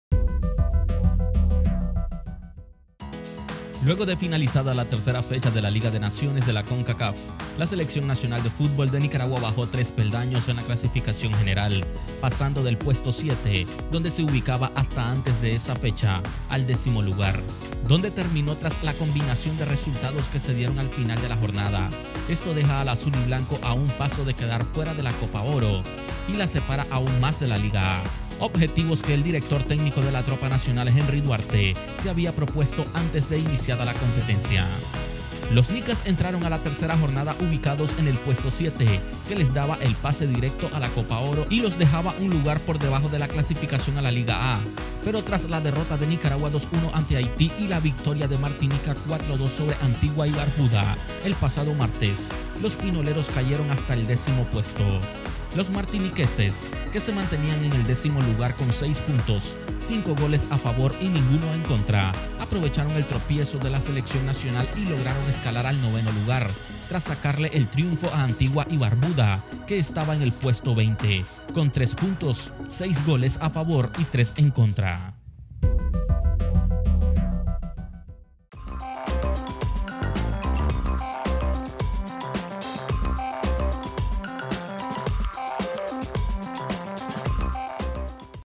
Selección de futbol de Nicaragua baja tres puestos en Liga de Naciones
liga de naciones de concacaf • seleccion de futbol de nicaragua • futbol de centroamerica • haiti • nicaragua • tv • video • noticias • noticiero • canal • medios

Luego de finalizada la tercera fecha de la Liga de Naciones de Concacaf, la selección nacional de futbol de Nicaragua bajó tres peldaños en la clasificación general, pasando del puesto siete, donde se ubicaba hasta antes de esta fecha, al décimo lugar.

¡Síguenos!